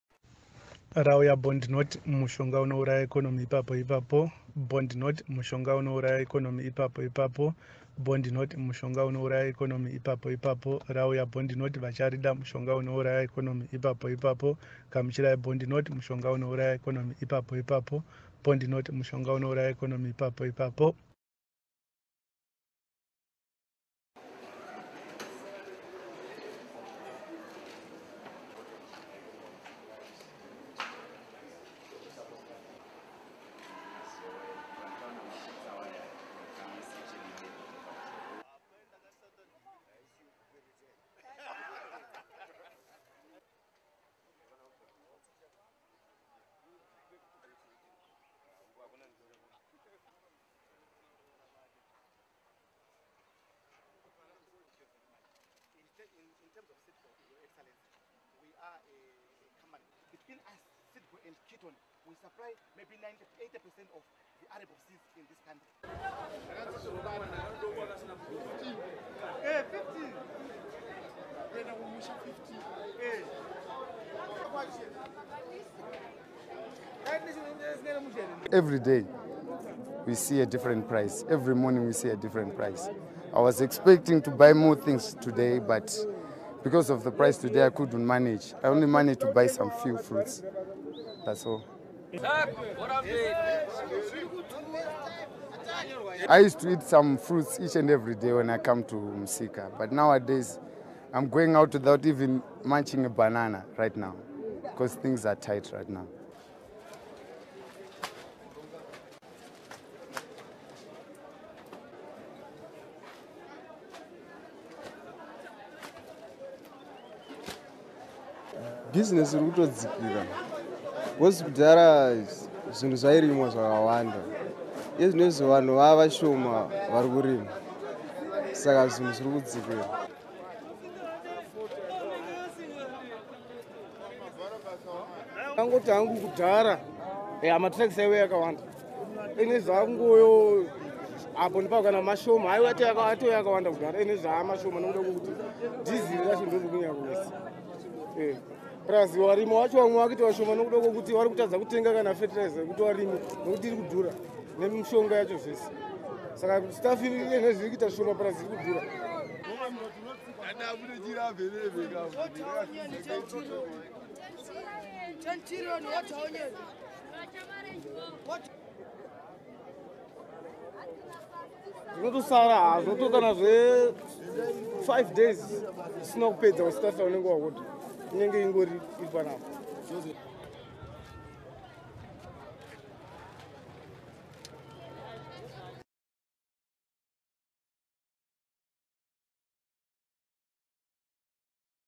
Zimbabwe RBZ Rauya Bond Zvamuchose
Finance Minister Patrick Chinamasa says the Reserve Bank of Zimbabwe will with immediate effect start the process towards issuance of bond notes as a legal tender in the country following a presidential edict to formalize its introduction.